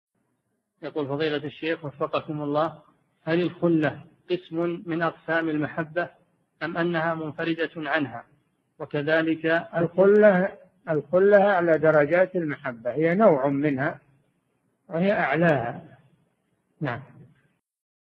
[415 /641] هل الخلة قسم من أقسام المحبة أم أنها منفردة عنها؟ الشيخ صالح الفوزان
[415 /641] السؤال: هل الخلة قسم من أقسام المحبة أم أنها منفردة عنها؟.
 الجواب: الخلة أعلى درجات المحبة هي نوع منها، وهي أعلاها.
السيرة الذاتية :
للاشتراك في قناة الشيخ صالح الفوزان
أسئلة وأجوبة من شرح كتاب العبودية ( 641 ) مقطع

للاستفسار والإقتراحات والدعم
رئيس الفريق
لمشروع كبار العلماء